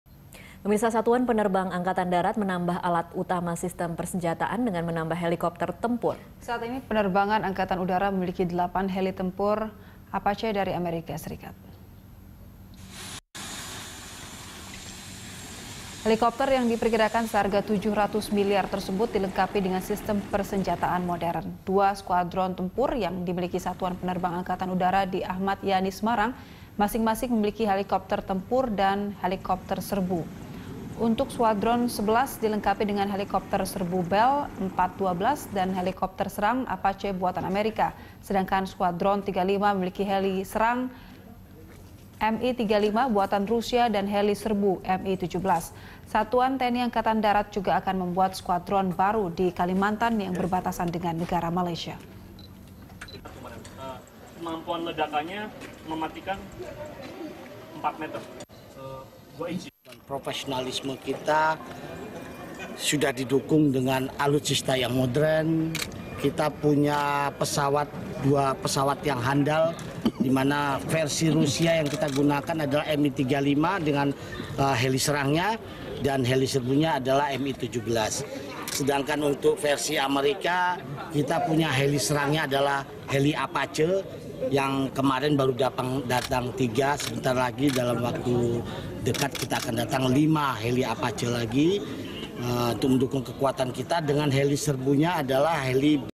TNI AD Tambah 8 Helikopter Tempur
Satuan Penerbang TNI Angkatan Darat menambah alat utama sistem persenjataan dengan menambah helikopter tempur. Saat ini penerbang AD memiliki delapan heli tempur Apache dari Amerika Serikat.

Facebook.com/BeritaSatuTV
Youtube.com/BeritaSatu